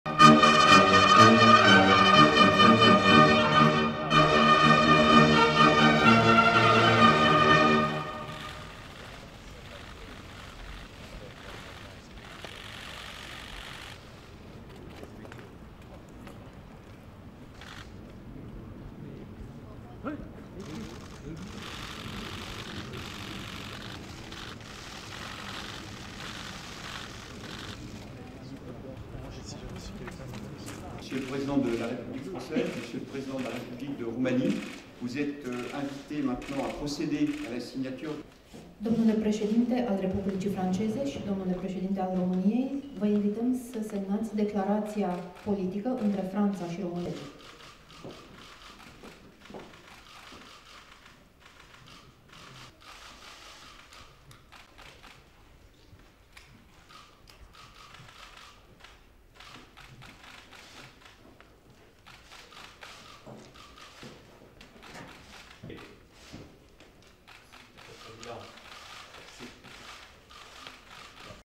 French and Romanian presidents meet at Elysee Palace in Paris
French President Macron welcomes his Romanian counterpart President Klaus Iohannis at the Elysee palace. IMAGES